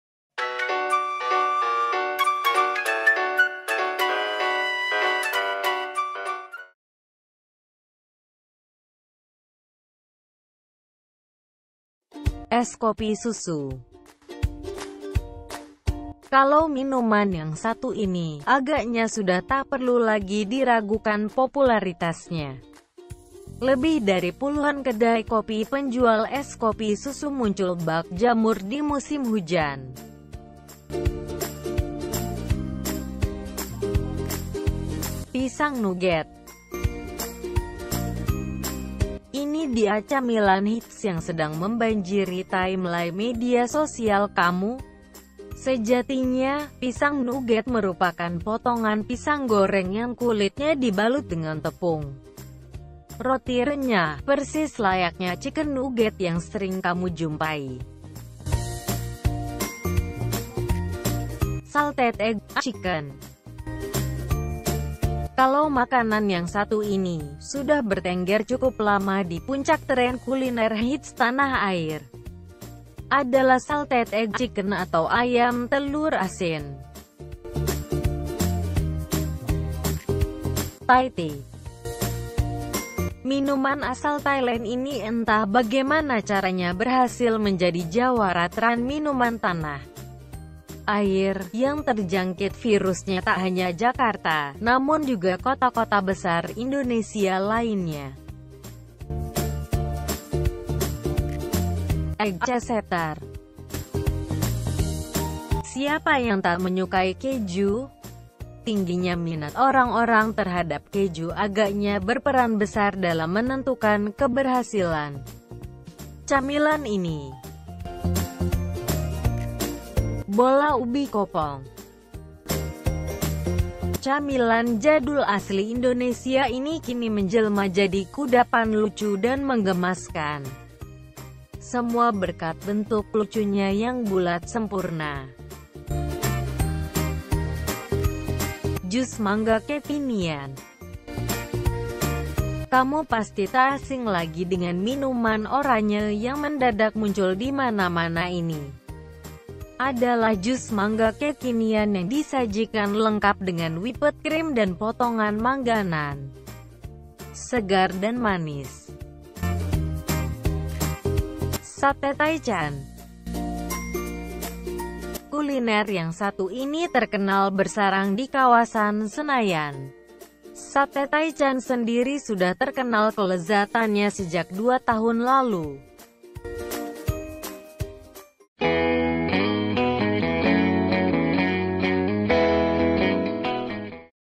8 Makanan Dan Minuman Kekinian Yang Sedang Hits Di Indonesia
8 Makanan Dan Minuman Kekinian Yang Sedang Hits Di Indonesia

Es Kopi Susu
Kalau minuman yang satu ini, agaknya sudah tak perlu lagi diragukan popularitasnya. Lebih dari puluhan kedai kopi penjual es kopi susu muncul bak jamur di musim hujan.

Pisang Nugget
Sejatinya, pisang nugget merupakan potongan pisang goreng yang kulitnya dibalut dengan tepung roti renyah. Persis layaknya chicken nugget yang sering kamu jumpai.

Salted Egg Chicken
Kalau makanan yang satu ini, sudah bertengger cukup lama di puncak tren kuliner hits Tanah Air. Adalah Salted Egg Chicken atau ayam telur asin.

Thai Tea
Minuman asal Thailand ini entah bagaimana caranya berhasil menjadi jawara tren minuman Tanah Air. Yang terjangkit virusnya tak hanya Jakarta, namun juga kota-kota besar Indonesia lainnya

Egg Cheesetart
Siapa yang tak menyukai keju?Tingginya minat orang-orang terhadap keju agaknya berperan besar dalam menentukan keberhasilan camilan ini.

Bola Ubi Kopong
Camilan jadul asli Indonesia ini kini menjelma jadi kudapan lucu dan menggemaskan. Semua berkat bentuk lucunya yang bulat sempurna.

Jus Mangga Kekinian
Kamu pasti tak asing lagi dengan minuman oranye yang mendadak muncul di mana-mana ini. Adalah jus mangga kekinian yang disajikan lengkap dengan whipped cream dan potongan mangga nan segar dan manis.

Sate Taichan
Kuliner yang satu ini terkenal bersarang di kawasan Senayan. Sate Taichan sendiri sudah terkenal kelezatannya sejak dua tahun lalu.